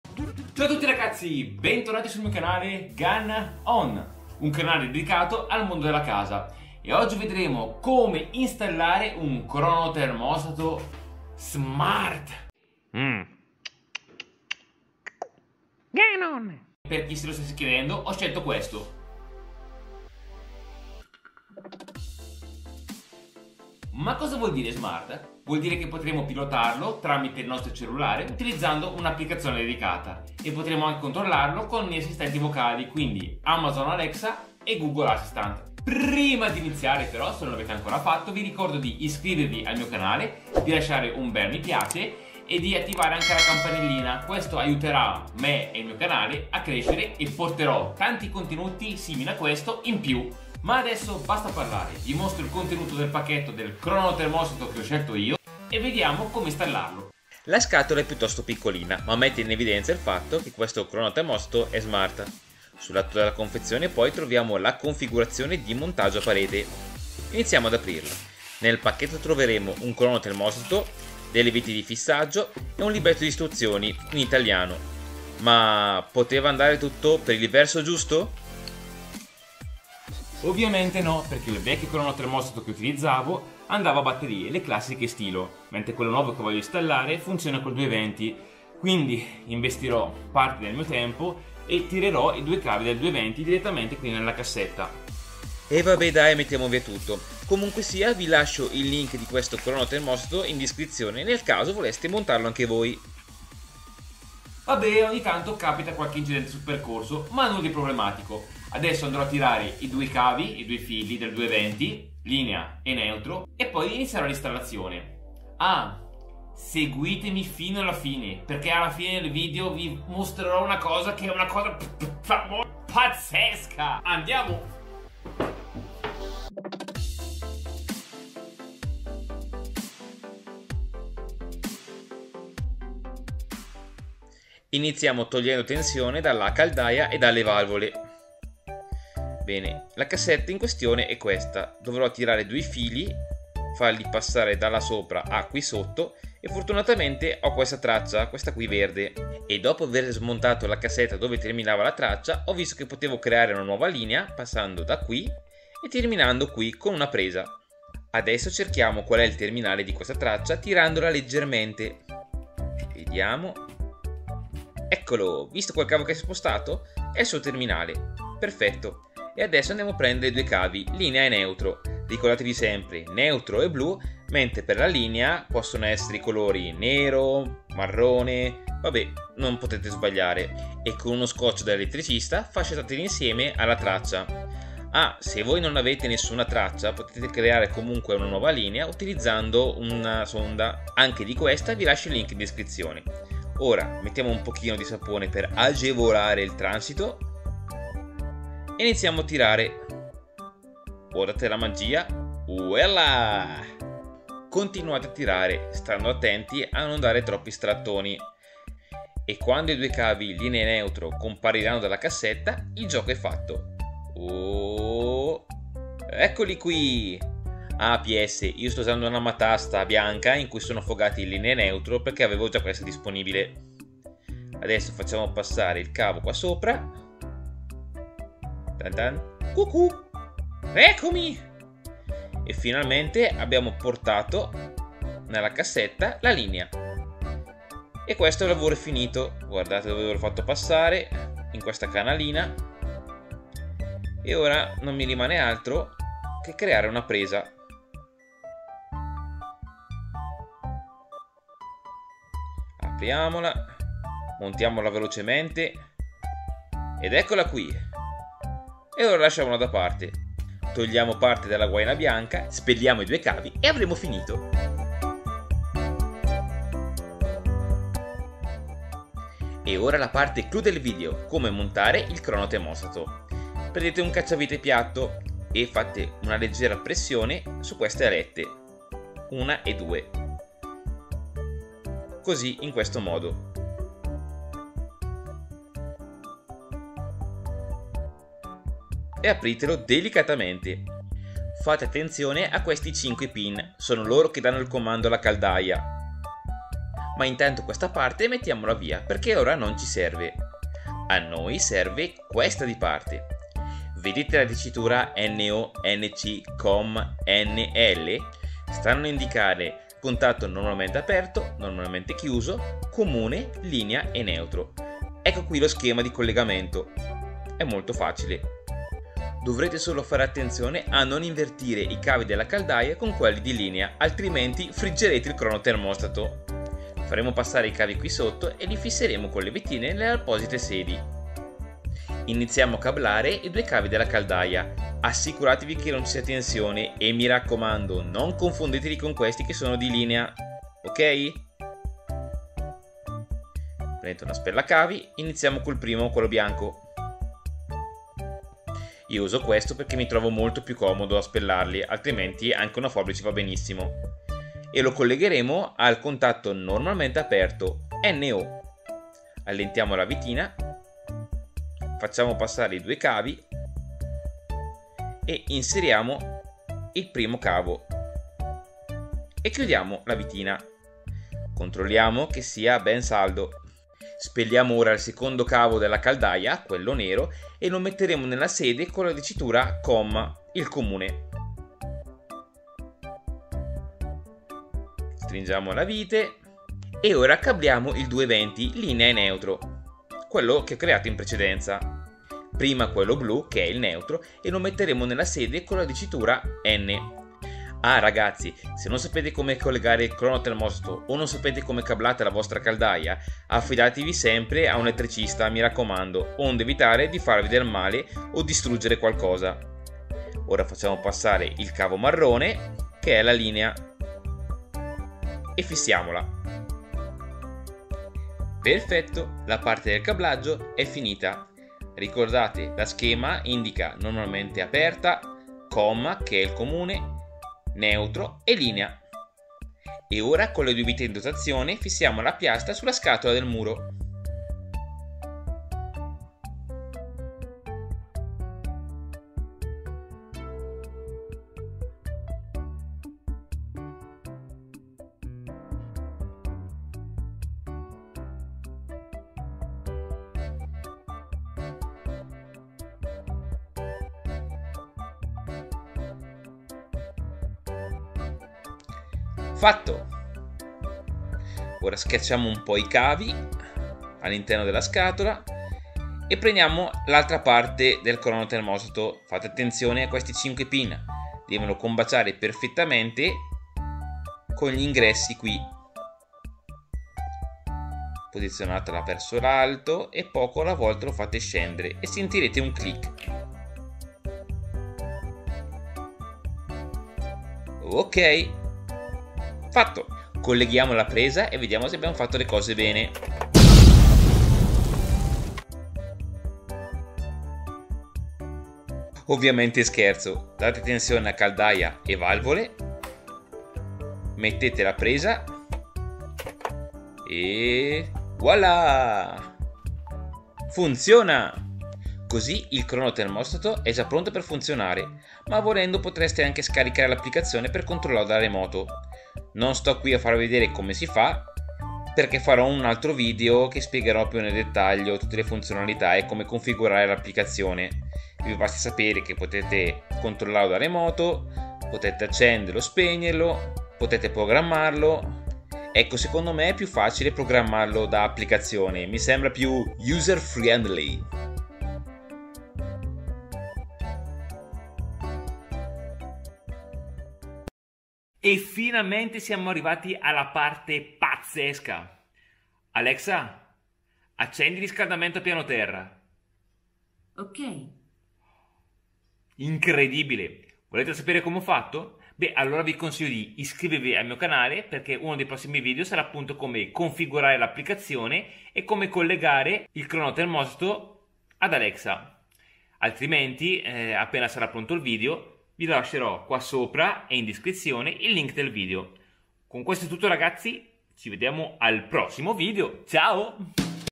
Come montare un Cronotermostato smart (Homecloud CTW)
Oggi vedremo come installare un cronotermostato Smart compatibile con Alexa! In più vedrete anche come creare la linea del 220v per alimentare questo dispositivo!

Link Cronotermostato

Link sonda(per creare una nuova linea)

Link spellacavi